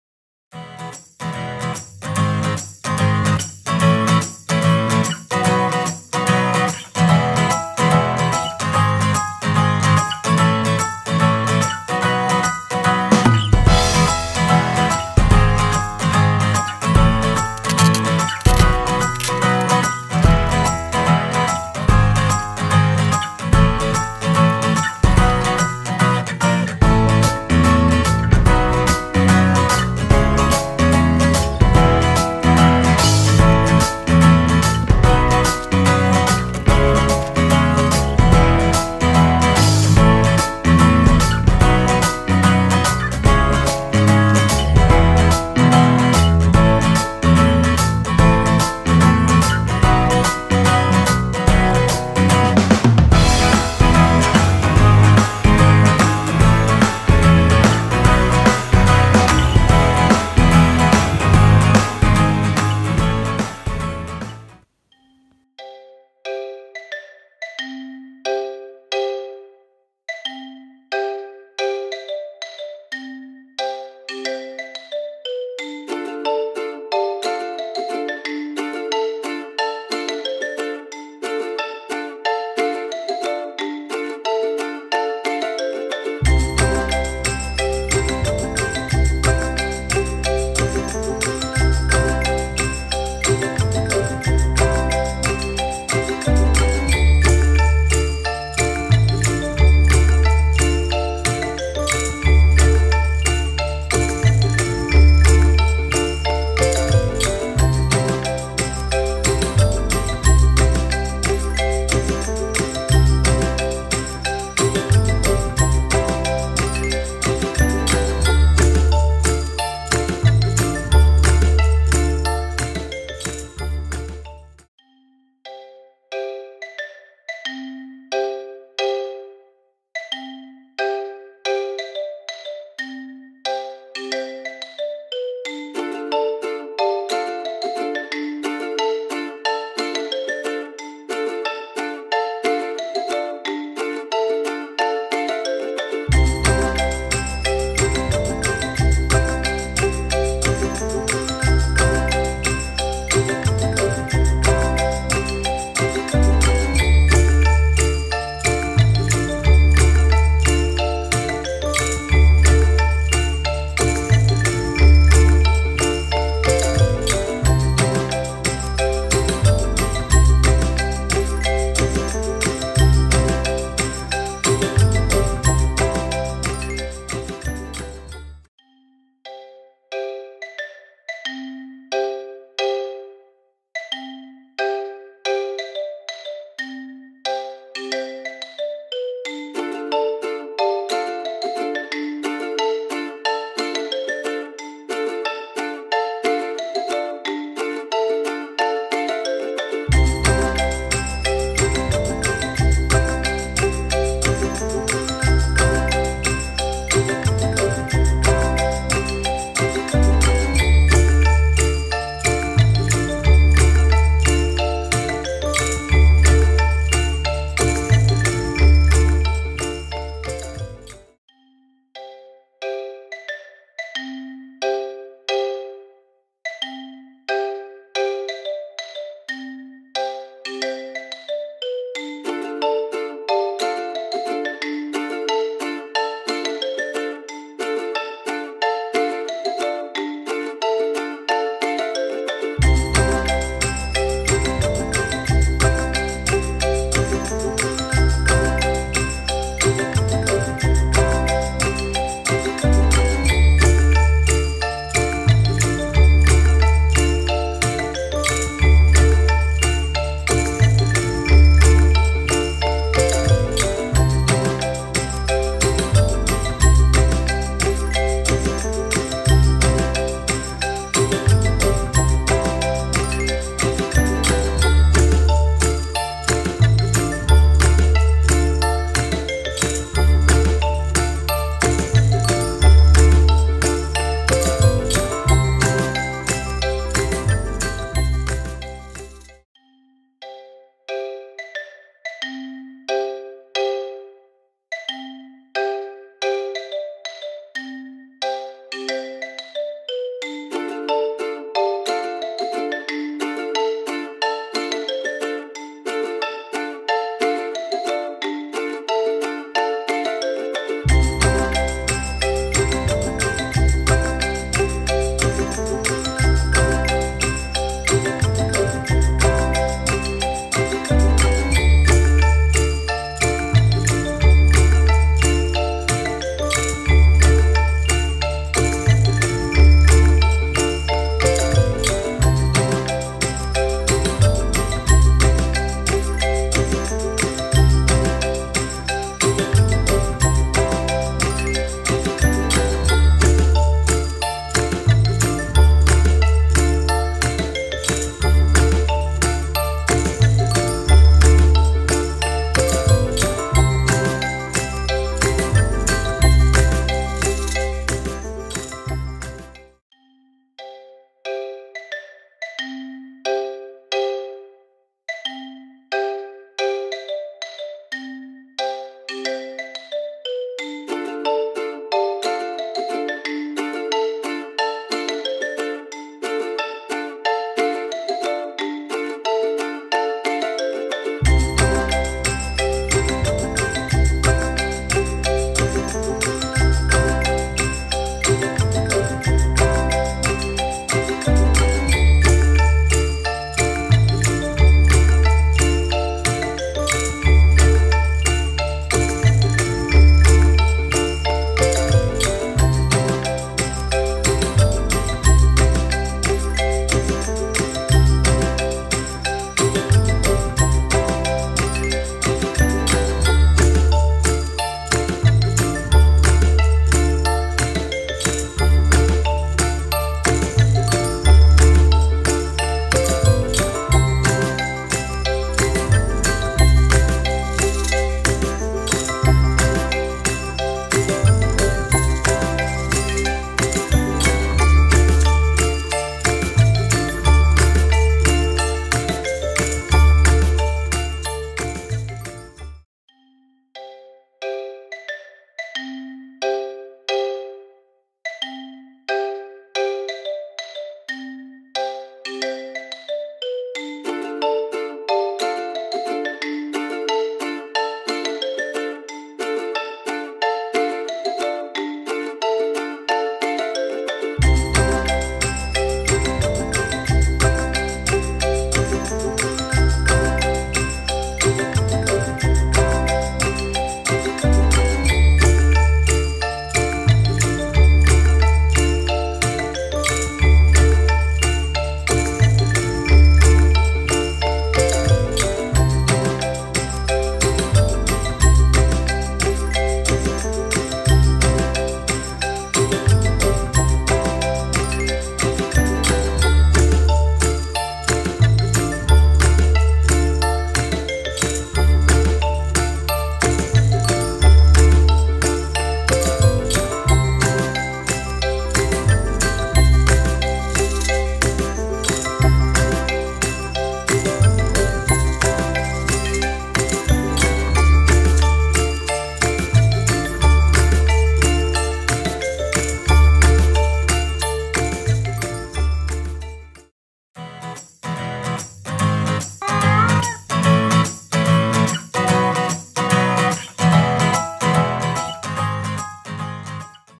Hamanaka Kit Needle Felting Tutorial - Pastel Nyankoro Kittens | Sweet Pea Dolls
Have lots of fun making these super cute needle felted kittens with our tutorial video for this lovely needle felting kit from Hamanaka, Japan!

This kit is available to purchase from Sweet Pea Dolls - Official Hamanaka Ambassador!